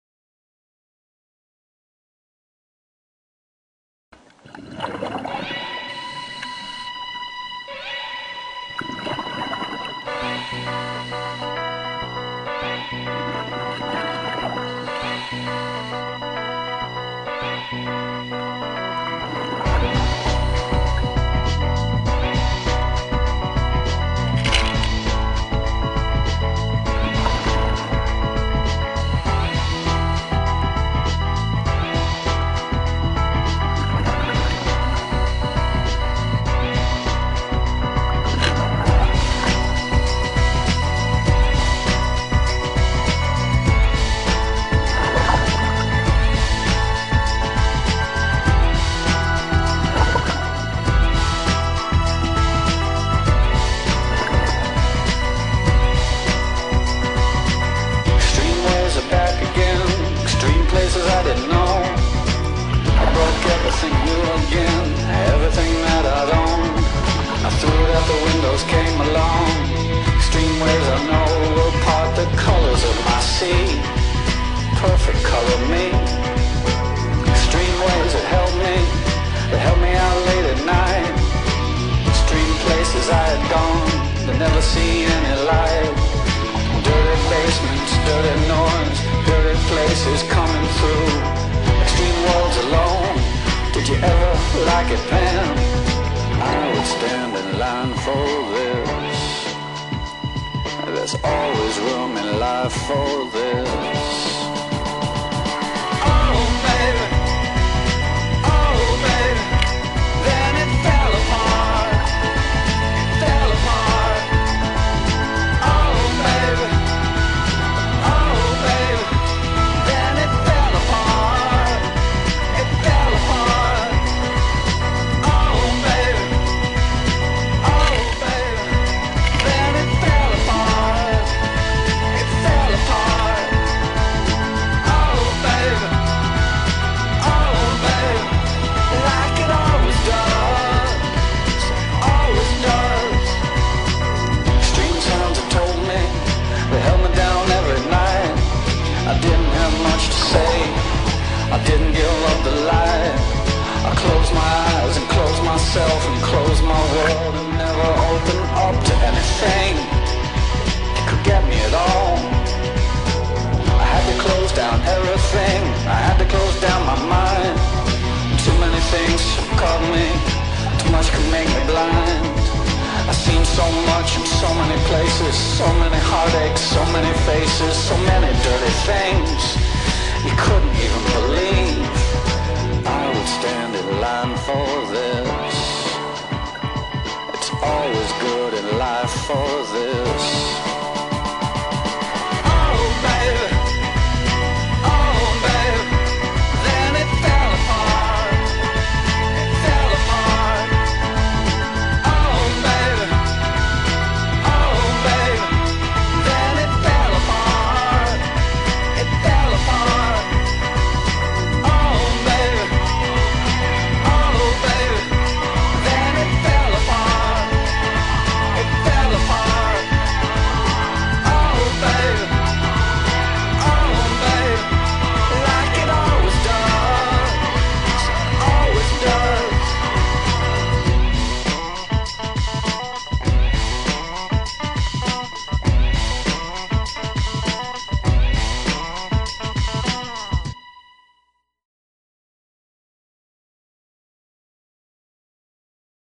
070930 Skills-SMB
An oldie from 2007 showing us practicing SMB deployment.
I can't watch this video now without thinking "Stow the lamp cable!"